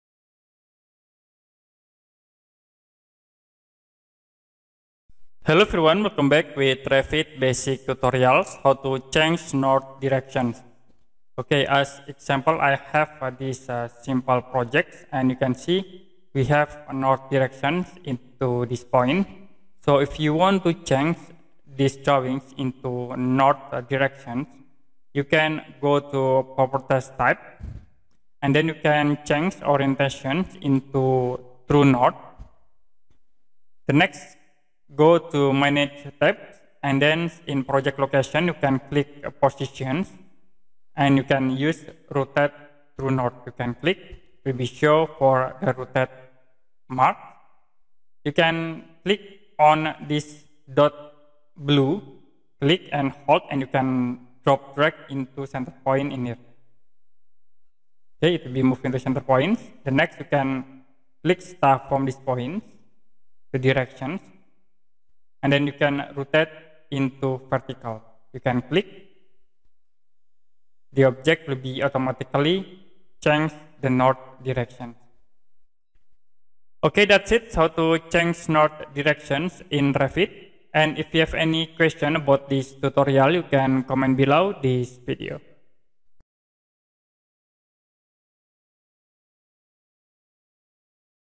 Revit How To Change North Direction Tutorial
Learn autodesk revit basic tutorial for beginner how to change north direction with rotate